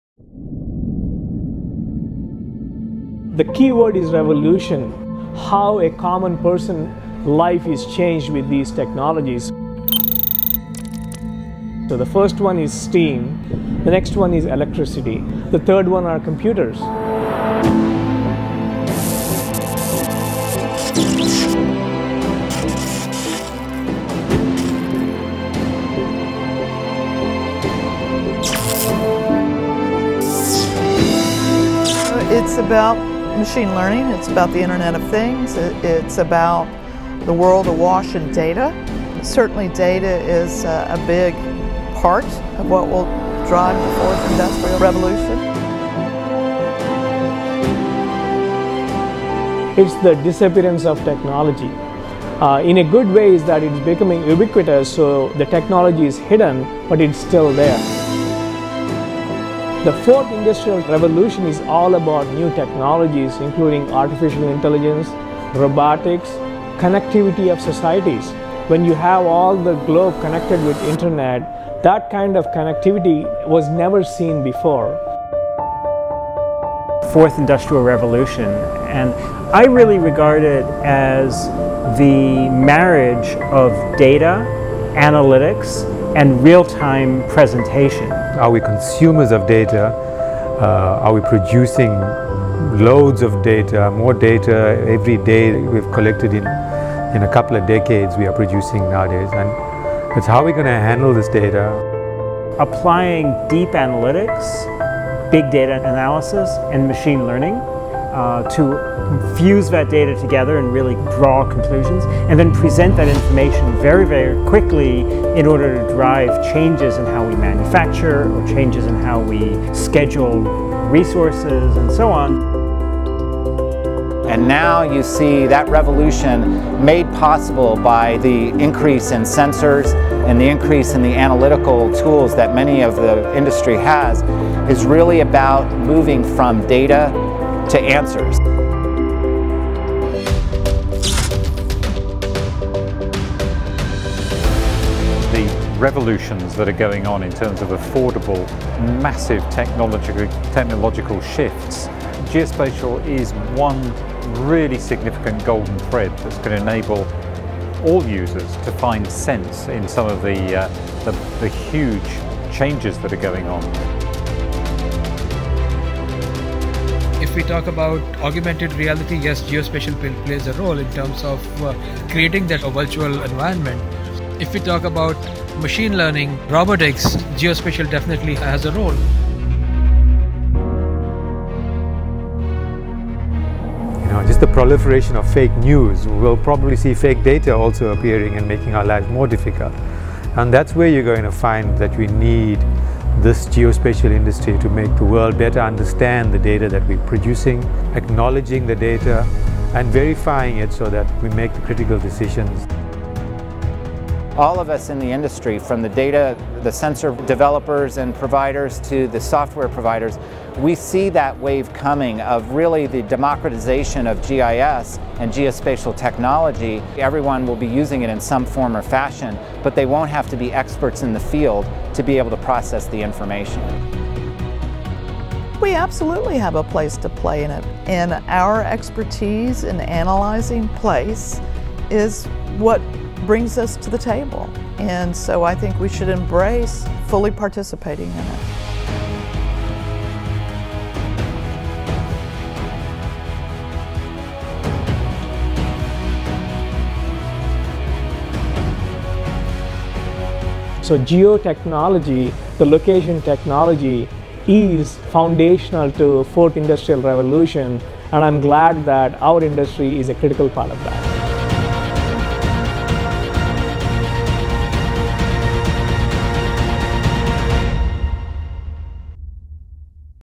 Geospatial - A Golden Thread in Fourth Industrial Revolution
Hear from the geospatial technology leaders and subject matter experts on how geospatial is turning ubiquitous in the era of fourth industrial revolution.